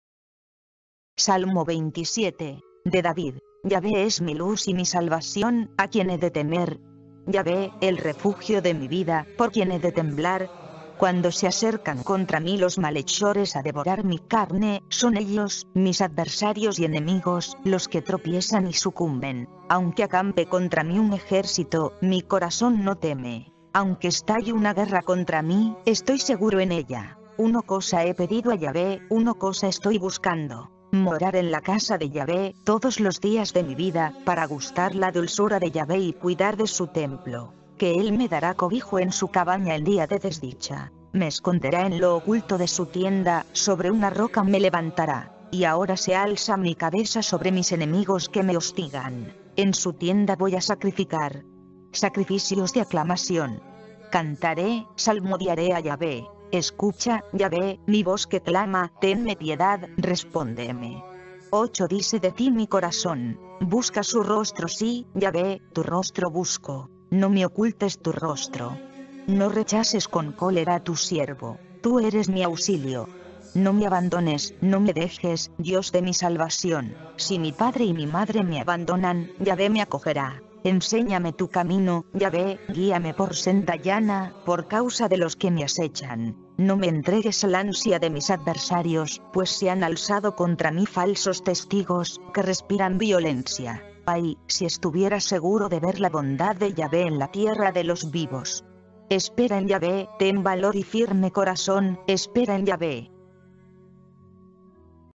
LA SANTA BIBLIA,VERSIÓN BIBLIA DE JERUSALÉN 1976, Salmos 27
Salmo 27

De David.
 Yahveh es mi luz y mi salvación, ¿a quién he de temer? Yahveh, el
refugio de mi vida, ¿por quién he de temblar?
 Cuando se acercan contra mí los malhechores a devorar mi carne,
son ellos, mis adversarios y enemigos, los que tropiezan y sucumben.
 Aunque acampe contra mí un ejército, mi corazón no teme; aunque
estalle una guerra contra mí, estoy seguro en ella.
 Una cosa he pedido a Yahveh, una cosa estoy buscando: morar en la
Casa de Yahveh, todos los días de mi vida, para gustar la dulzura de
Yahveh y cuidar de su Templo.
 Que él me dará cobijo en su cabaña en día de desdicha; me
esconderá en lo oculto de su tienda, sobre una roca me levantará.
 Y ahora se alza mi cabeza sobre mis enemigos que me hostigan; en
su tienda voy a sacrificar. sacrificios de aclamación. Cantaré, salmodiaré a
Yahveh.
 Escucha, Yahveh, mi voz que clama, ¡tenme piedad, respóndeme!
8 Dice de ti mi corazón: «Busca su rostro.» Sí, Yahveh, tu rostro
busco:
 No me ocultes tu rostro. No rechaces con cólera a tu siervo; tú eres
mi auxilio. No me abandones, no me dejes, Dios de mi salvación.
 Si mi padre y mi madre me abandonan, Yahveh me acogerá.
 Enséñame tu camino, Yahveh, guíame por senda llana, por causa
de los que me asechan;
 no me entregues al ansia de mis adversarios, pues se han alzado
contra mí falsos testigos, que respiran violencia.
 ¡Ay, si estuviera seguro de ver la bondad de Yahveh en la tierra de
los vivos!
Espera en Yahveh, ten valor y firme corazón, espera en Yahveh.